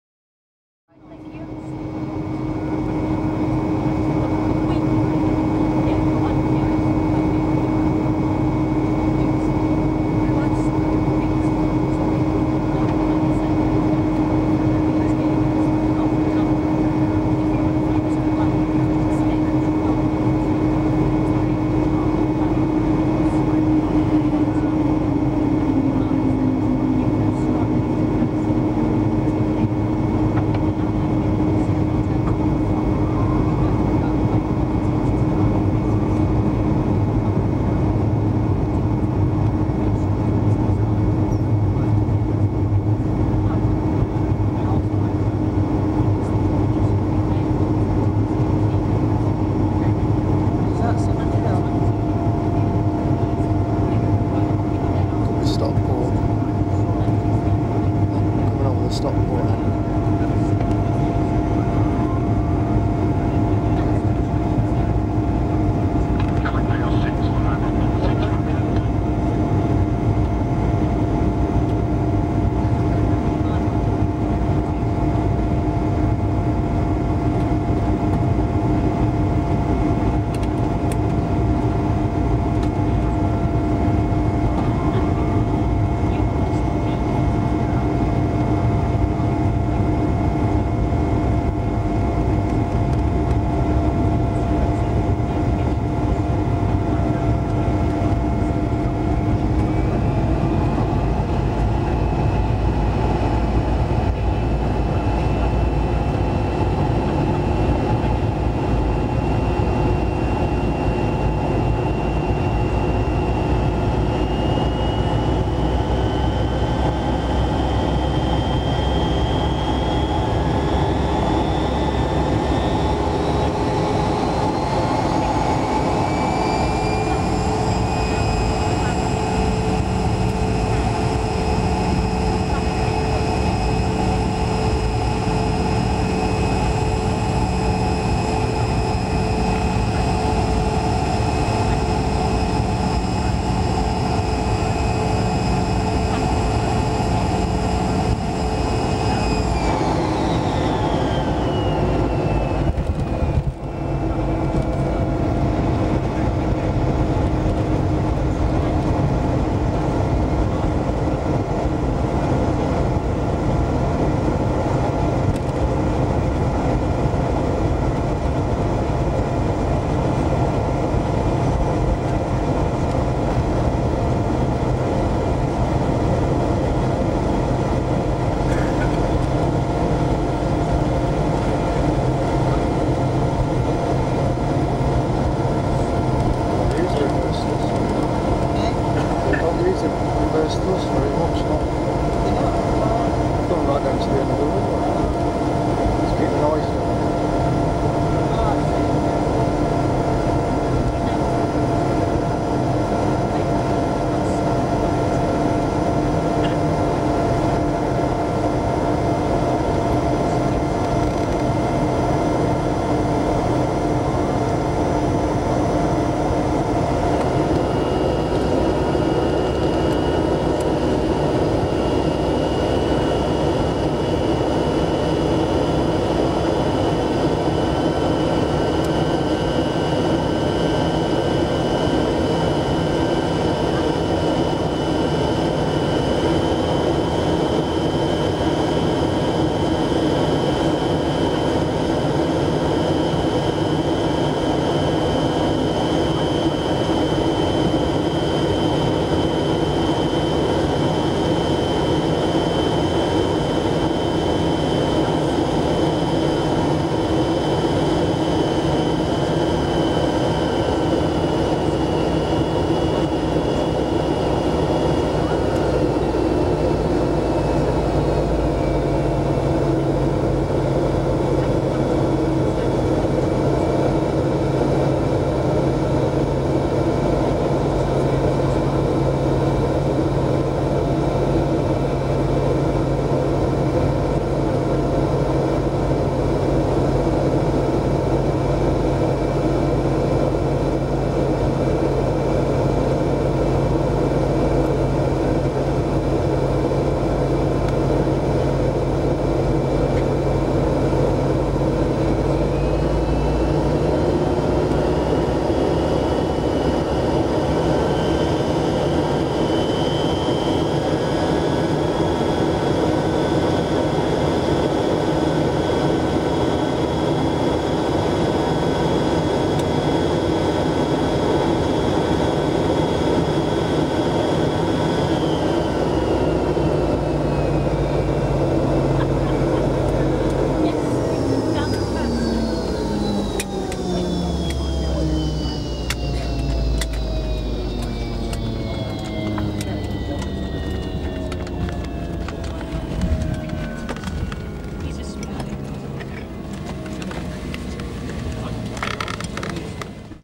Dan-Air London BAC One Eleven. Landing (wing view)
Window view from a Dan-Air One Eleven landing, taxing to stand and engine shut-down.Landing at Manchester Airport, departed airport was London Gatwick. Filmed in 1992